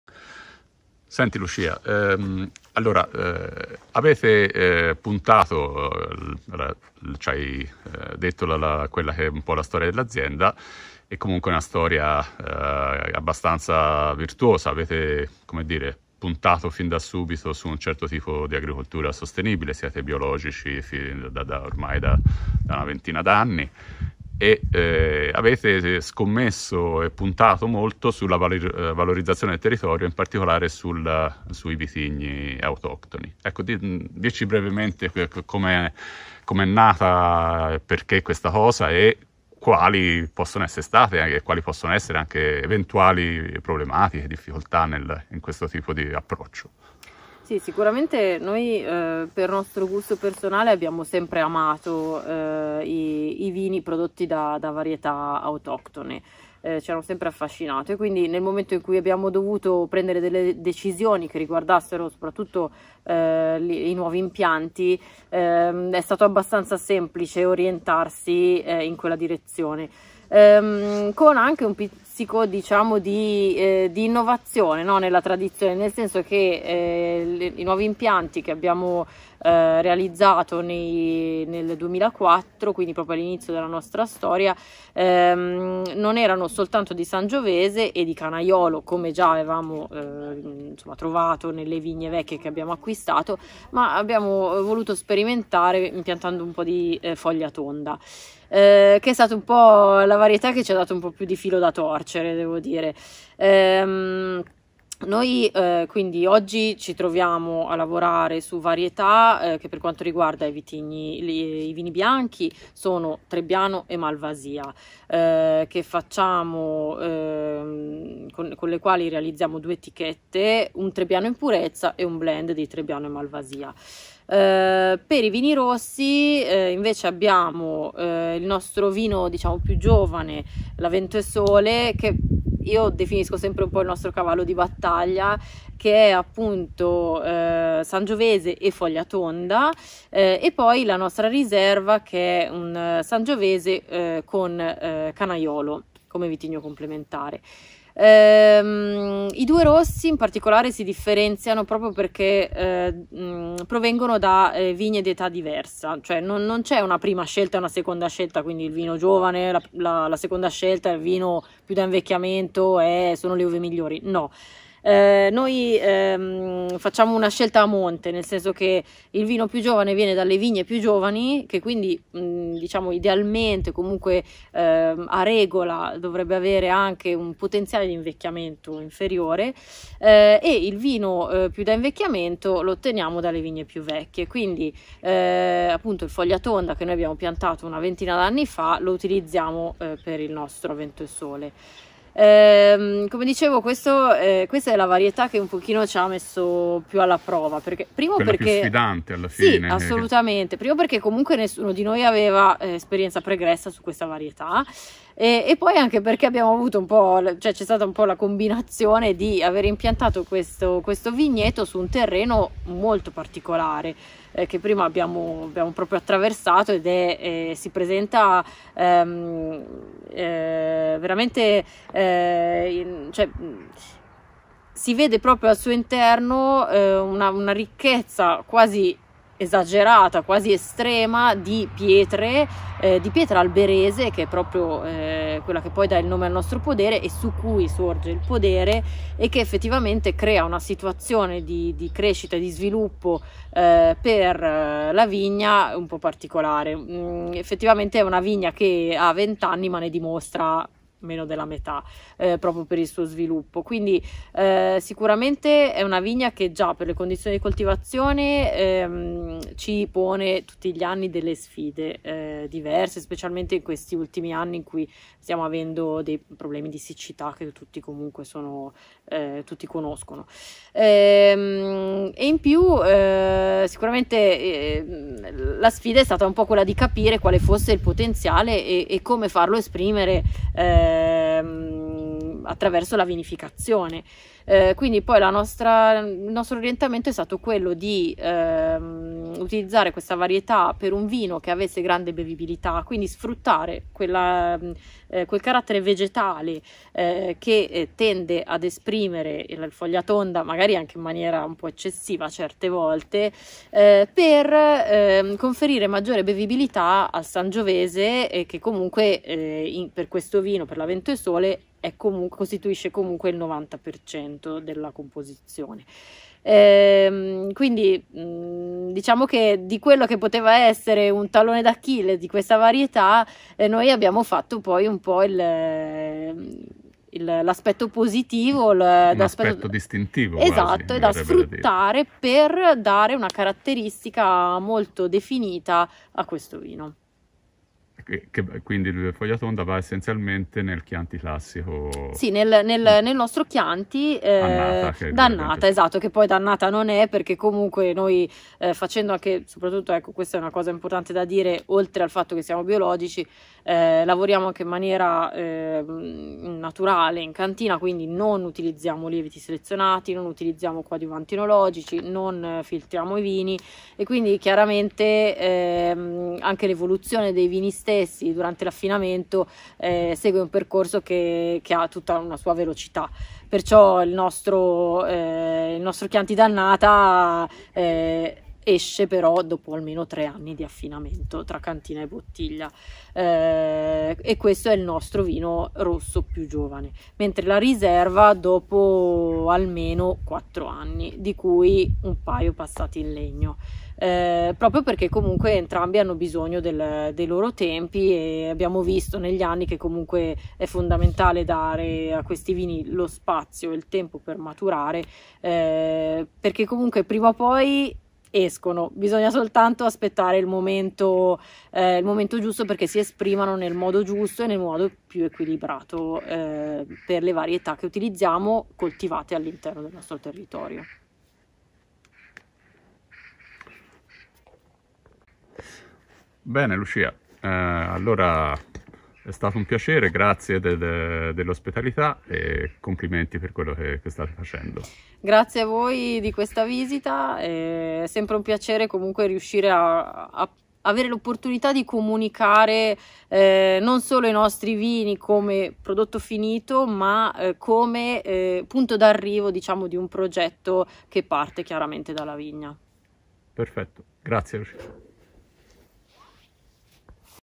Podere Alberese, Toscana: Slow Wine 2023
Visita in azienda per la guida Slow Wine 2023 presso Podere Alberese, Toscana. Intervista a cura di Fausto Ferroni